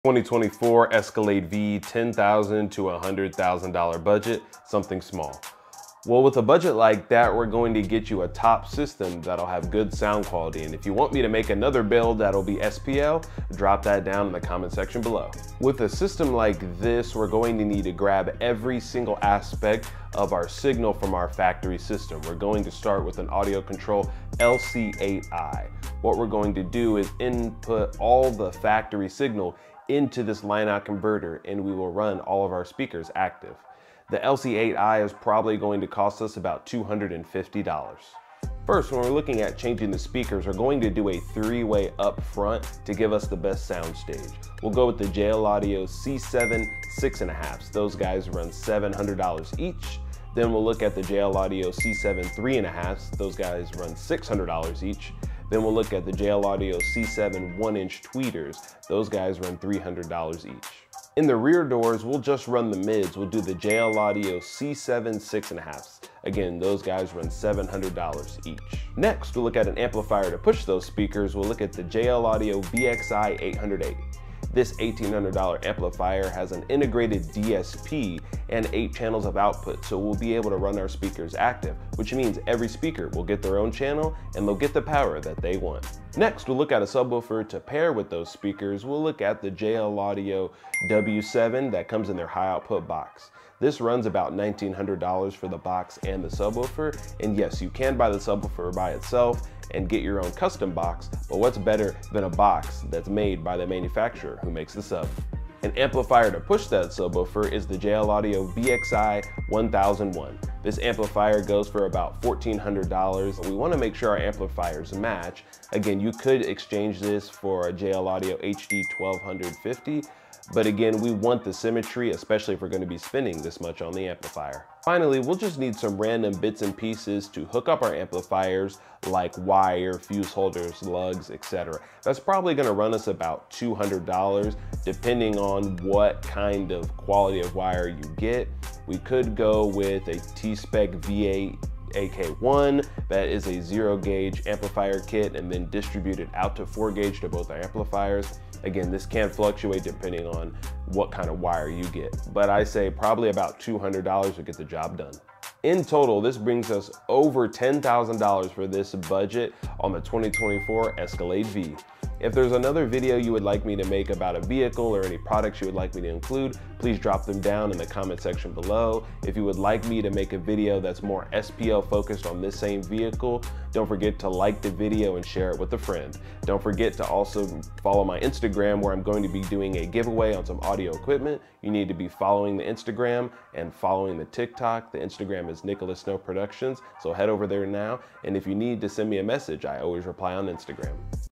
Best SQL Audio System For A 2024 Escalade V! What's Your Favorite SQL Brand?
🚀 Best SQL Audio System for a 2024 Cadillac Escalade V 🚀

In this video, we showcase the ultimate Sound Quality Loud (SQL) setup for the 2024 Cadillac Escalade V! 🔊 Prepare to experience booming bass, crisp highs, and unmatched clarity for the perfect ride. Whether you're cruising or showing off at events, this system will blow you away!

- Premium speakers for maximum clarity 🎶
- Powerful amplifiers to drive the system 🔥
- Deep, punchy bass with seamless subwoofer integration 🎧
- Perfect integration with the Escalade's sleek interior 🚙

Follow me for more awesome builds and reviews!